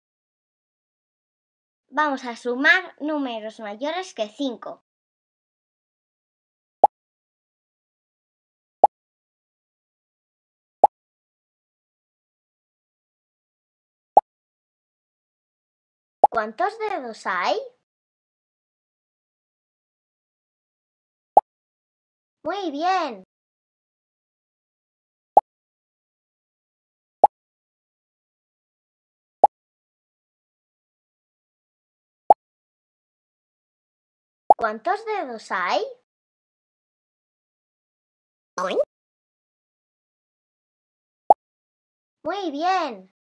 ABN - Suma números mayores que 5 (12-18)
En este juego se presenta y practica el procedimiento para hacer sumas de números mayores que 5 con los dedos, así como la tabla de sumar.